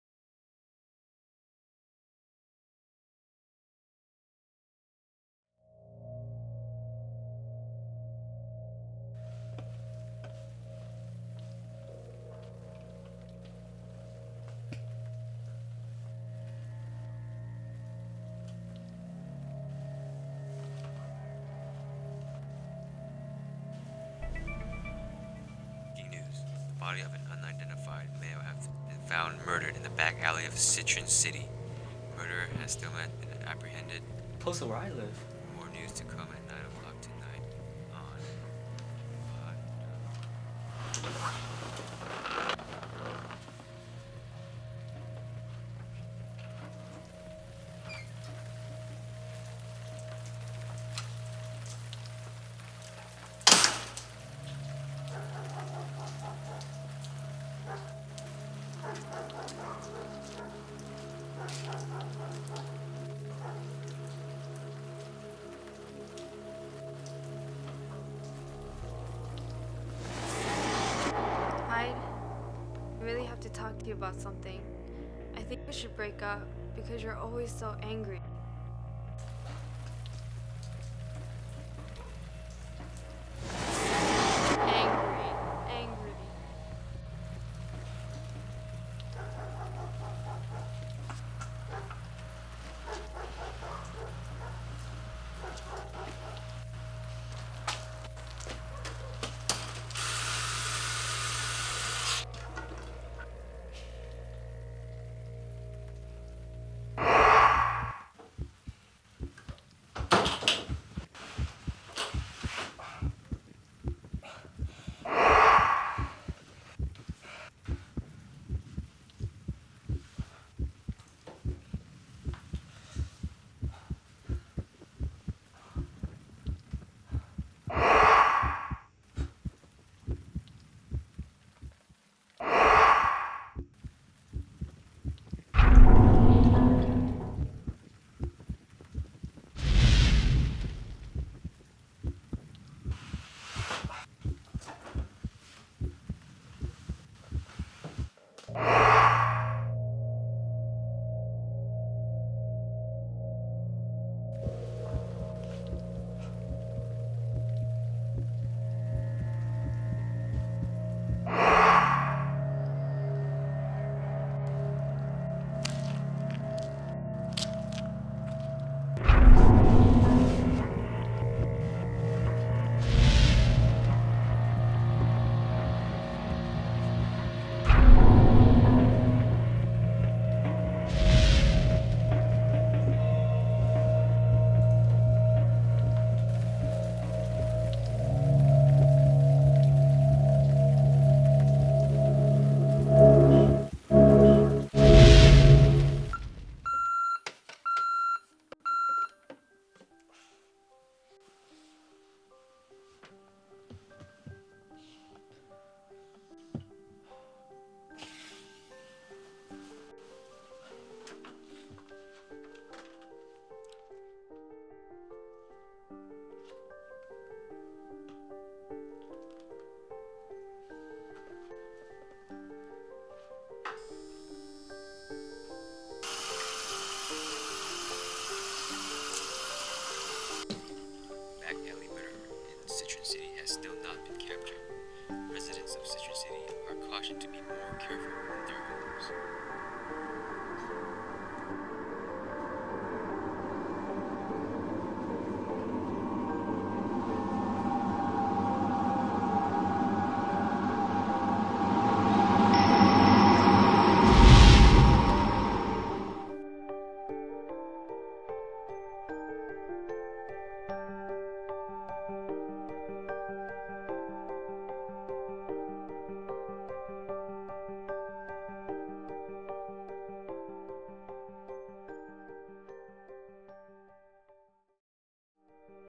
Short Film Project: Oscillate
Short Film Project for Film class. Requirements: Under 4 minute 30 secs. Actor: Gouji Yu as Hyde, Christie Kondo as the girlfriend, and "Chilli" as the dead victim. Help from David Ortiz in sound effects.